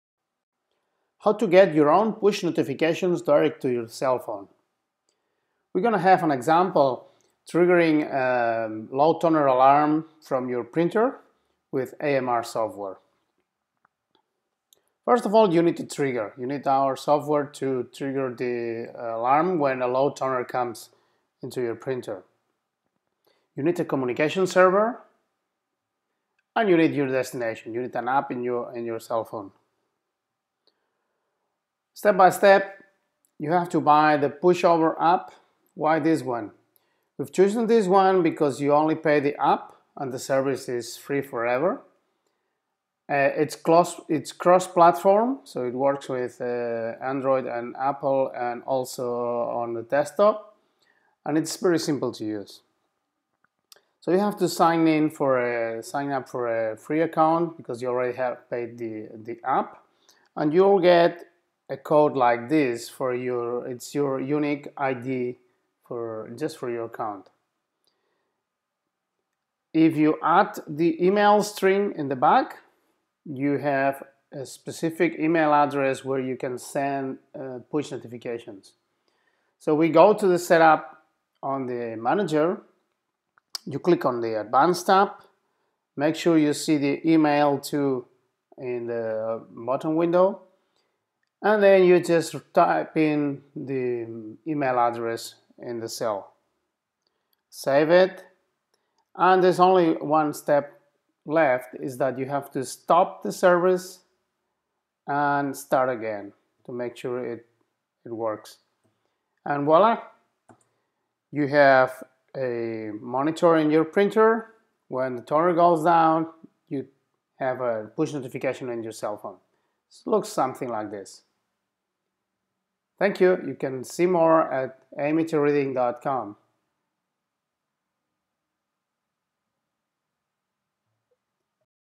How to setup your own push notifications
You can setup push notifications very easily.
Let's see an example with AMR printer monitoring software + Pushover push notifications.

- Download the Pushover app (4,99€)
- Sign up a free account.
- Get your 30 digit unique ID
- Add the email string @api.pushover.net
- Stop and start the service
- You're done.